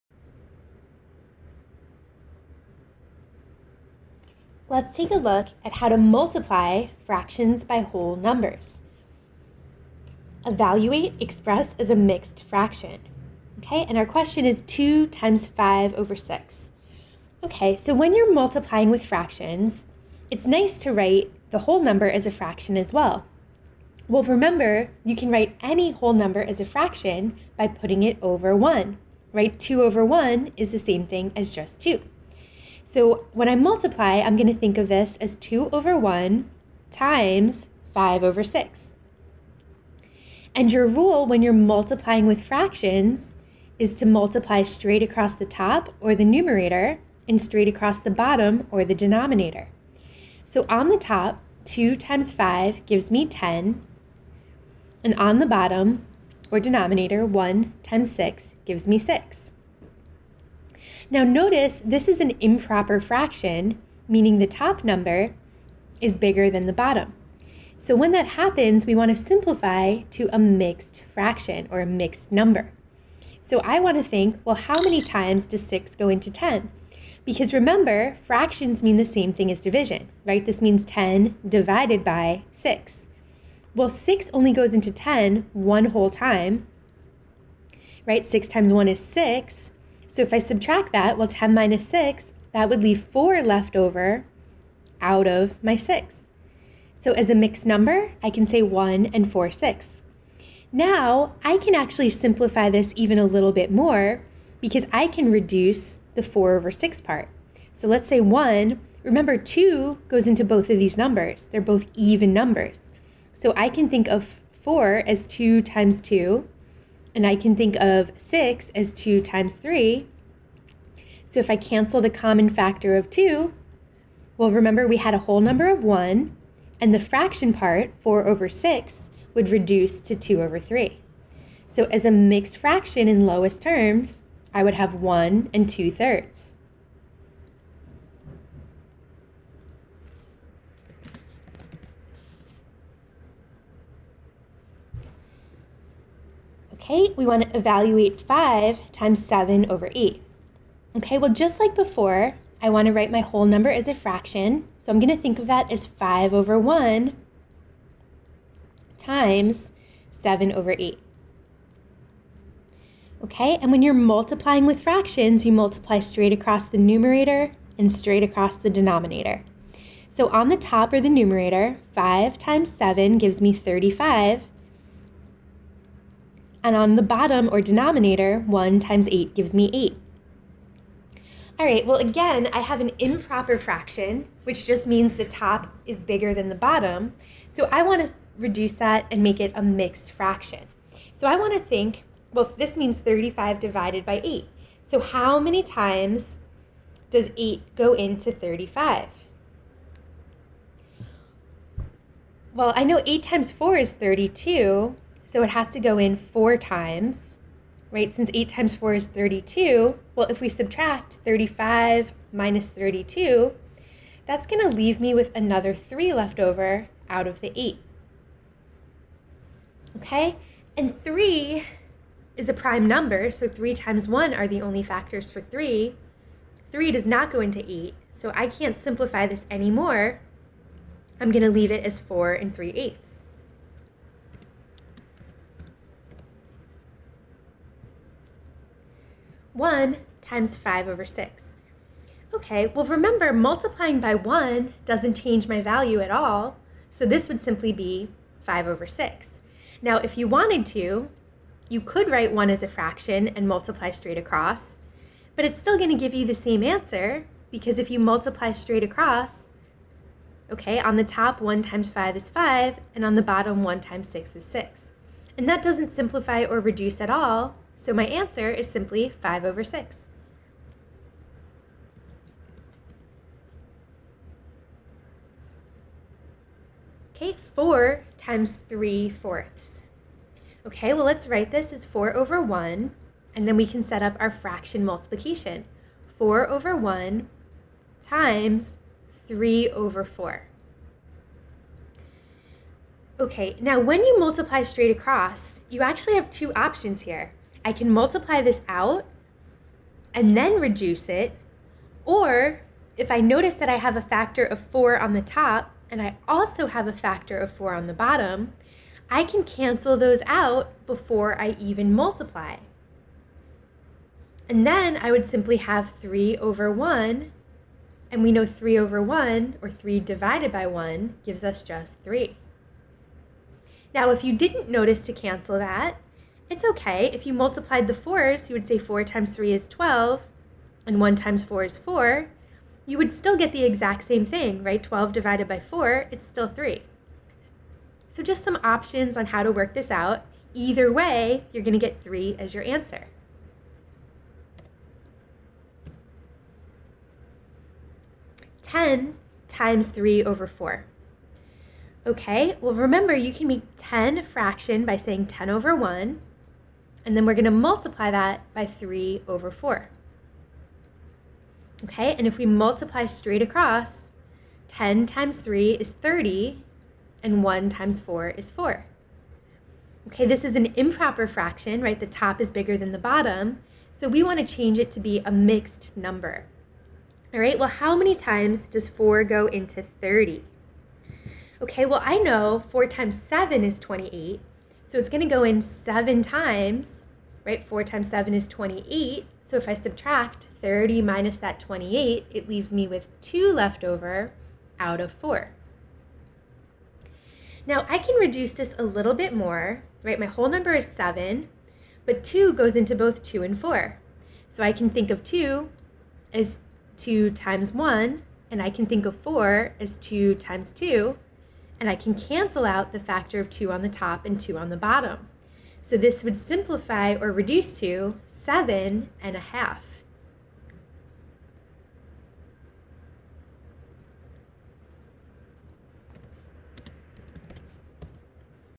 7.104 Multiply Fractions by Whole Numbers
Multiply Fractions by Whole Numbers
Solve fraction multiplication equations with whole numbers.